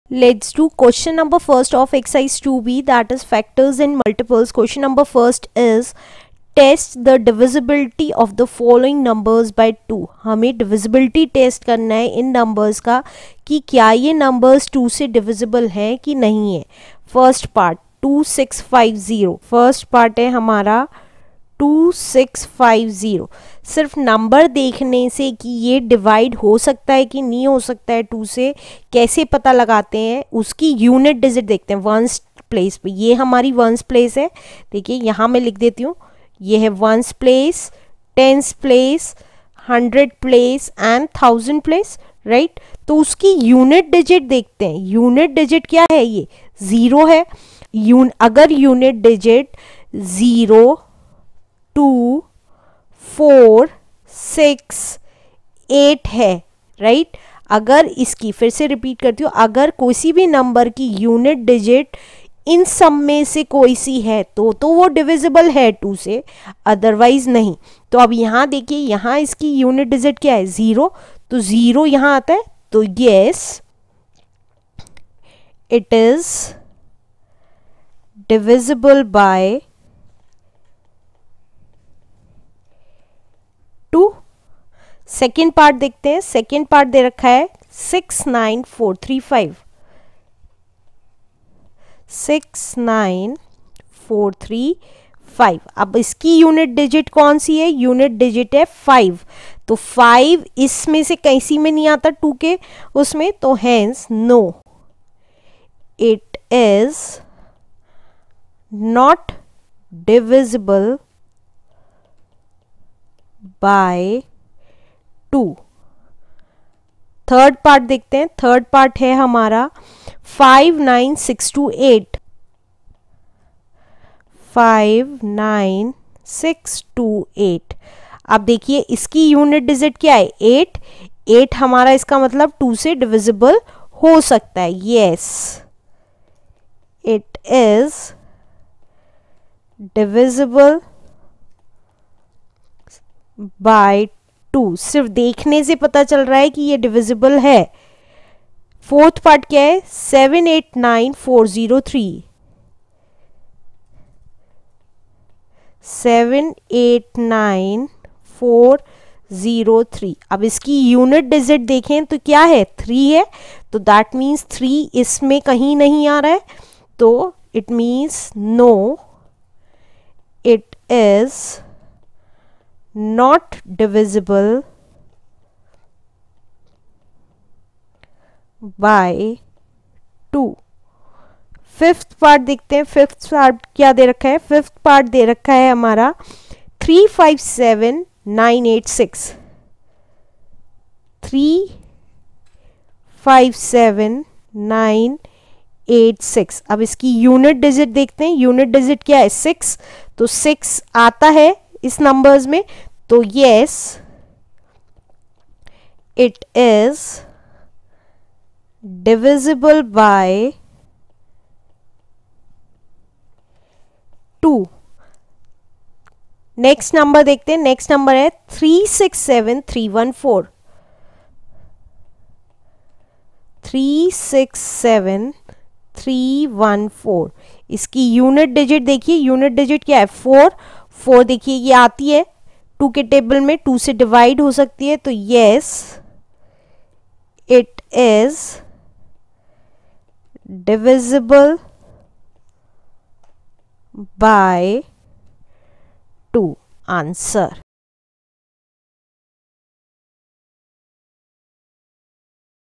Factors and Multiples | Class 6 Exercise 2B Question 1 | RS Aggarwal | Learn Maths
Class - 6
Chapter - 2 - Factors and Multiples
Exercise - 2B
Question - 1
Welcome to Learn Maths. This video will solve the Question 1 of exercise 2B. Please share your queries and feedback in comments section.

Regards,
Garima Agarwal